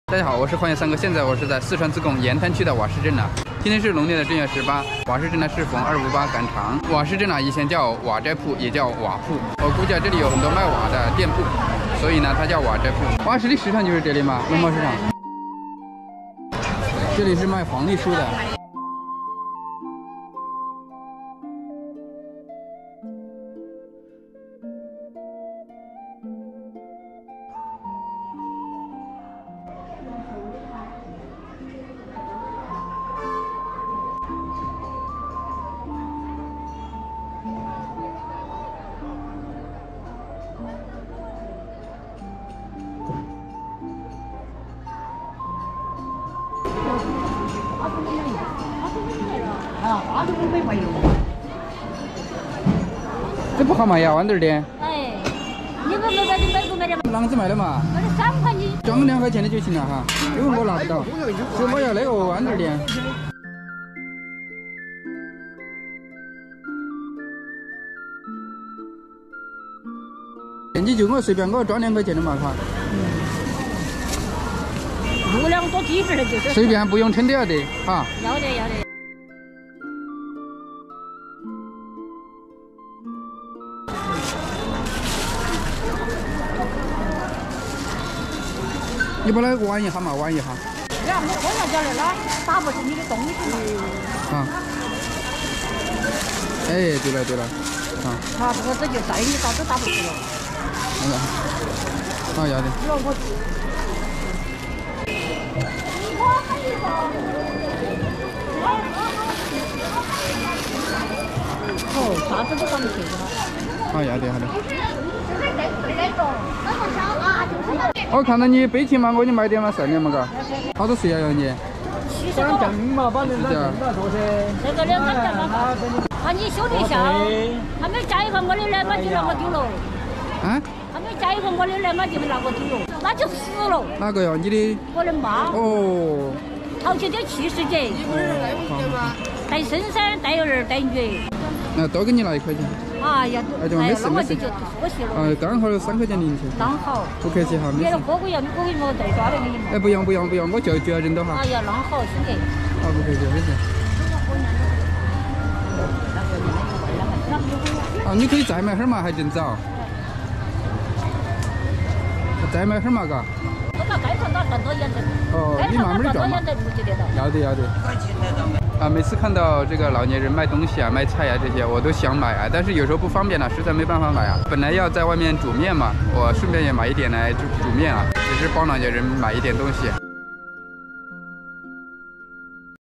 四川自贡瓦市赶场，豌豆尖3元一斤，给从小没妈的婆婆买了6两
简介:户外独行侠 分享有趣的风景、美食！
我会每周更新一到两个影片！